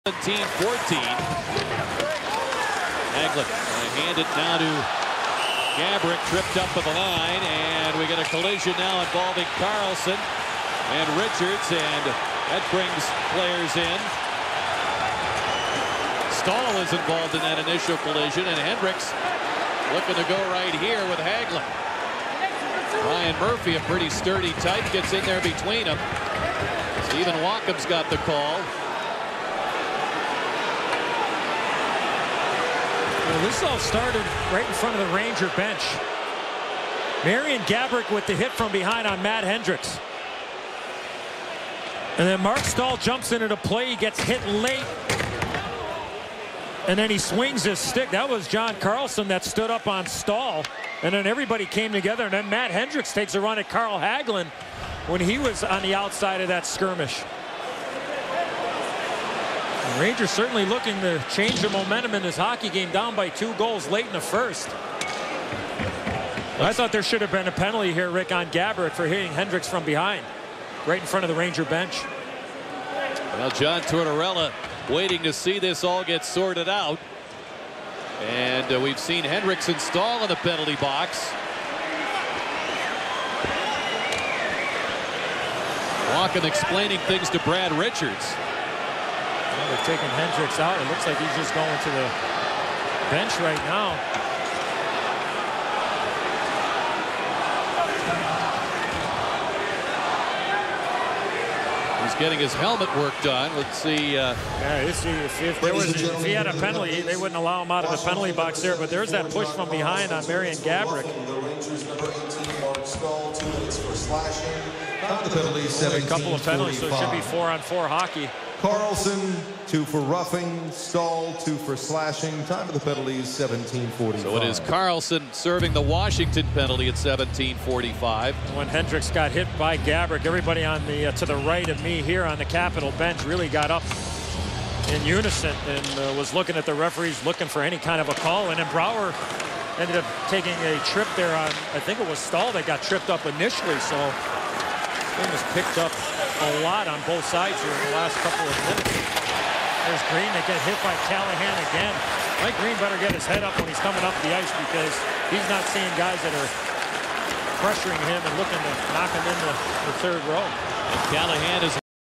Scrum near end of 1st. Washington Capitals vs NY Rangers Game 2 4/30/12 NHL Hockey
Scrum near end of 1st.

Washington Capitals vs NY Rangers Game 2 4/30/12 NHL Hockey.

US National TV: NBC Sports Network
Play by Play: Rick Peckham
Inside the Glass: Eddie Olczyk